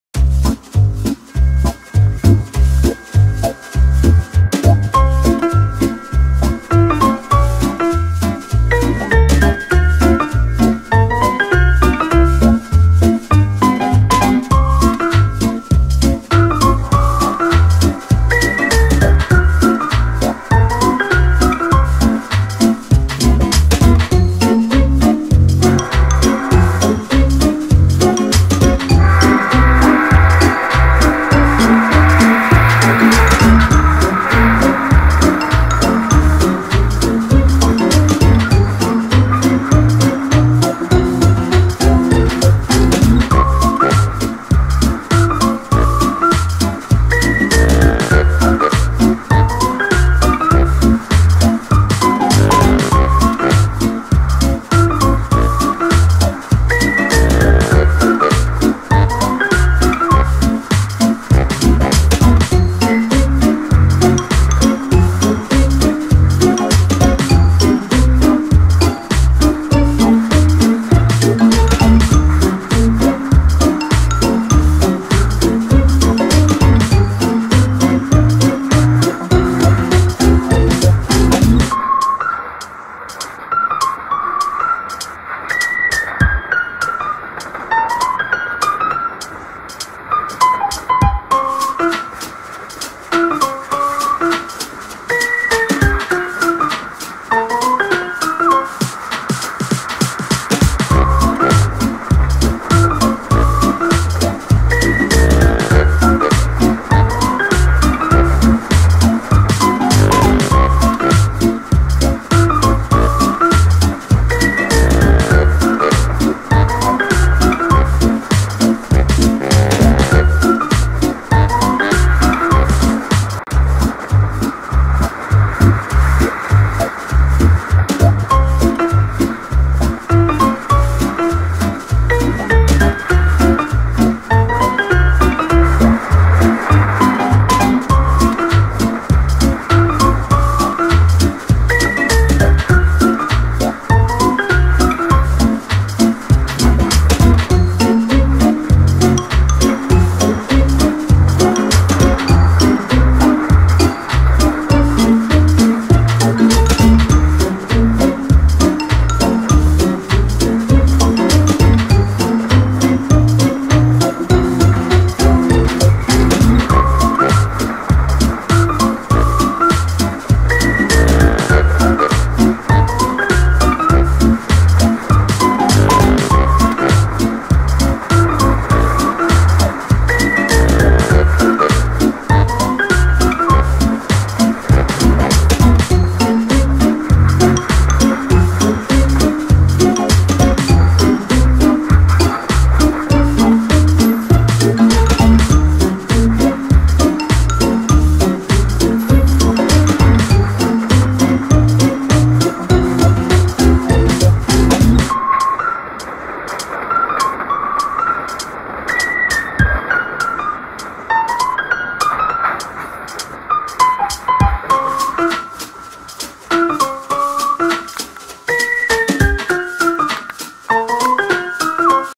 A CSX Train goes through Glenolden while I play fitting music
So its a Saturday, I was bored, just completed a school project for TV II class, I found a track I could loop, found a really long CSX train clip. take a wild guess what I did. Enjoy this wasting 3 minutes of your time :3